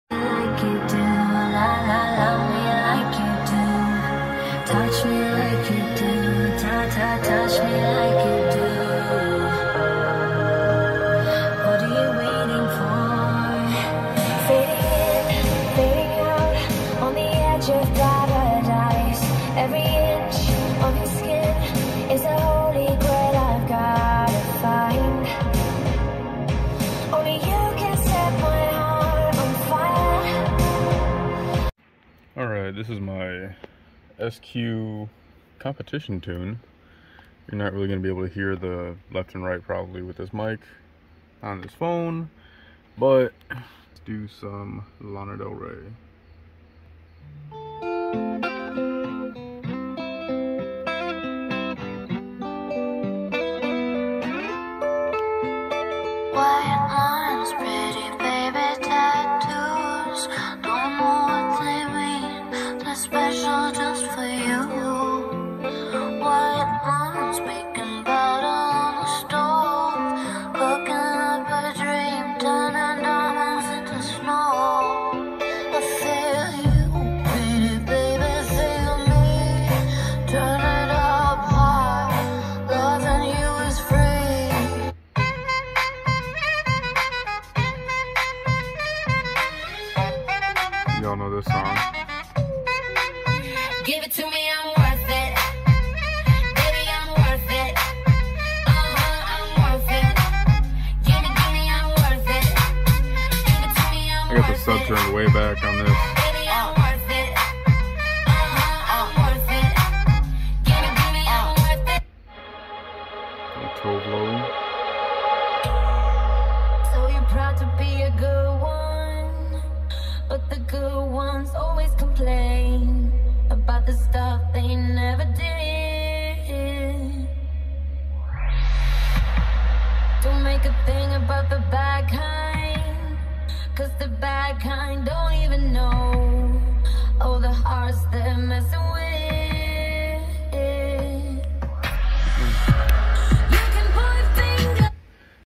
Sound Quality tune demo with the Arc PS8-50
ACX-BT1
Arc Audio PS8-50 amp/dsp
Soundstream Reference 1000SX
Scanspeak D3004/602000 tweeters
PHD FB6.1 midbass
Adire Audio 12KH501d2 in 1.2ft³gross
Knukonceptz Kolossus 1/0 power/ground for main power
db Link Maxxkore 10ga speaker wire
db Link Maxxkore 4ga power/ground wire for amps
Amazon 100A main circuit breaker
Amazon terminal busbars
Amazon 14p terminal strip
KnuKonceptz inline MANL fuse holders to amps
LifePo4 lithium cells for second battery